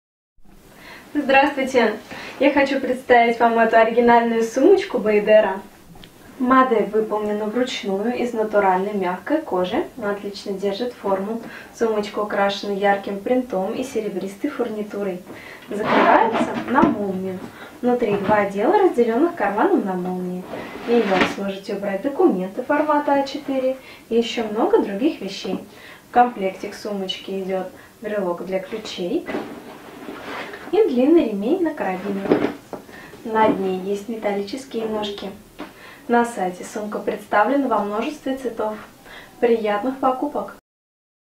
Сумка Baiadera MAIA FARFALLE 02 brown multicolor
Мечтаете создать оригинальный образ? Это возможно с отличной сумкой от известного бренда Baiadera. Эксклюзивная дизайнерская модель из высококачественной натуральной кожи, расписанной вручную. Находка для модниц, которые хотят выделяться в толпе. Такая сумка подчеркнет вашу изысканность и индивидуальность, а также сделает ваш образ ярким и неповторимым. Ее размеры позволят уместить все самое необходимое для деловой женщины: косметику, документы и деловые бумаги, телефон, небольшую книгу, портмоне и даже компактный зонт. Украшена серебристой фурнитурой и фирменным брелоком на карабине. Закрывается на молнию, носится на сгибе руки или на плече.
Внутри: два отдела, разделенных карманом на молнии. На задней стенке карман для документов на молнии, на передней - два кармана для мелочей.